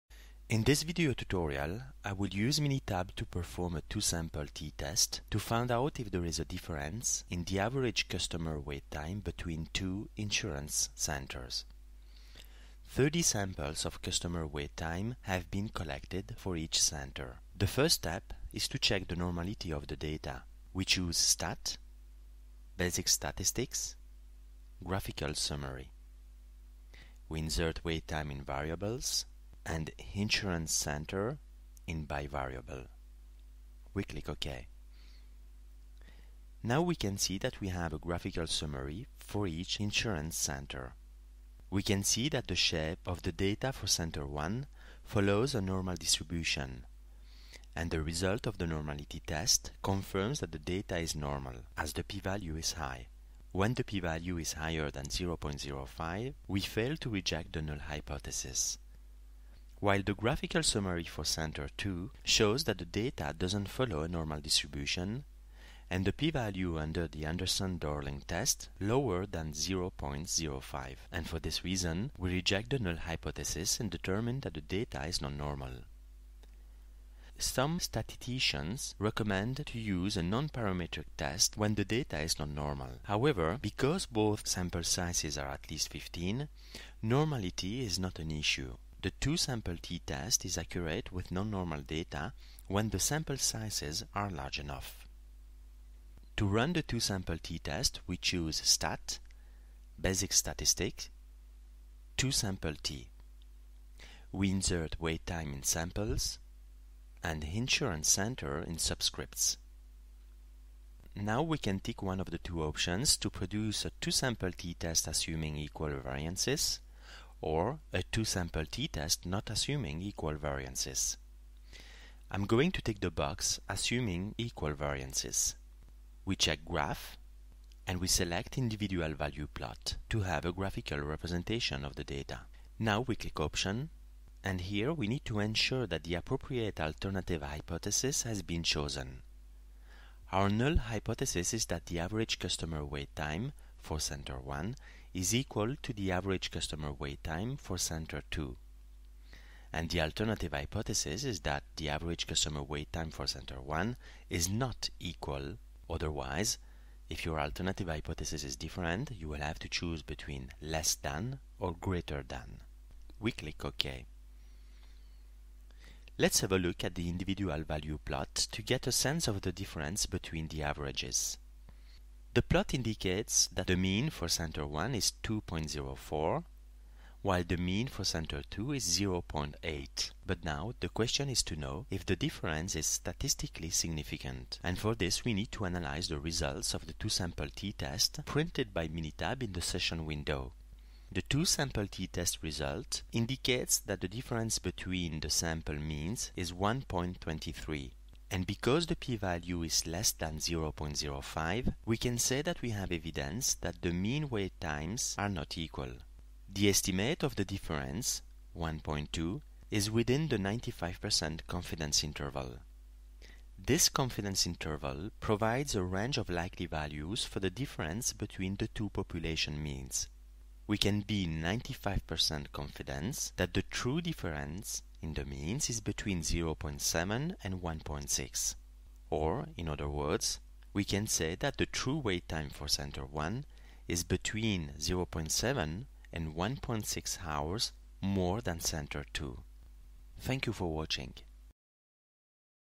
Two-sample t-test in Minitab
A two-sample t-test helps determine whether two population
means are different.